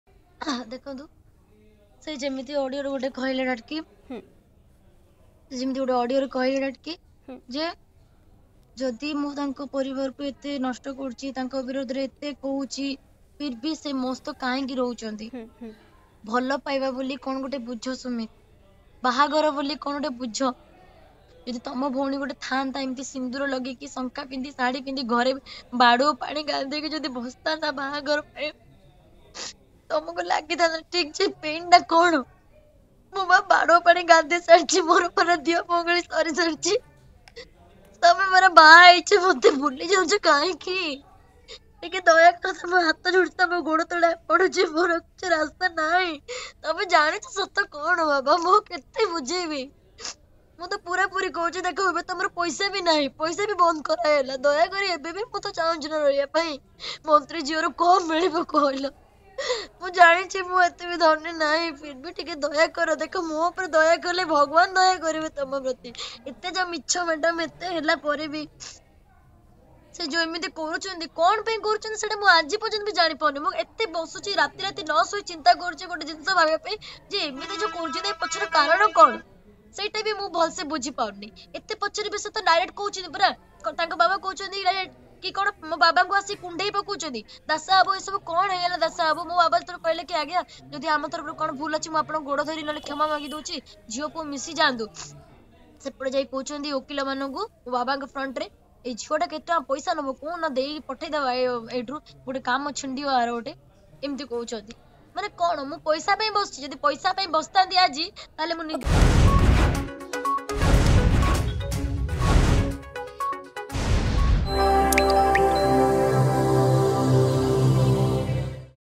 Berhampur Marital Dispute: Sumit is yet to Understand What is love and Marriage, alleges Dimple
Dimple alleges Sumit is yet to Understand What is love and Marriage.